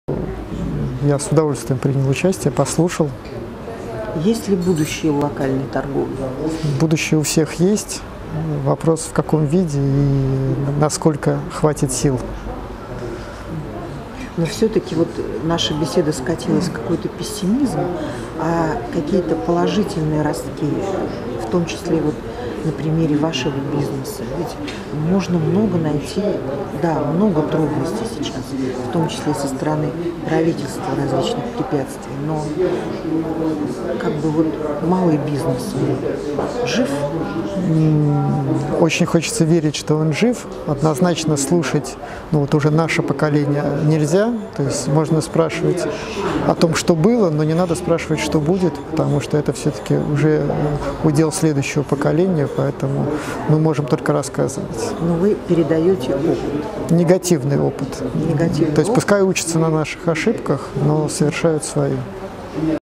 Александр Мышинский (глава группы компаний "РеалЪ") на бизнес-завтраке
29 октября состоялось второе заседание делового клуба "Свое Дело"
в формате бизнес завтрака. Почетным гостем стал Александр Мышинский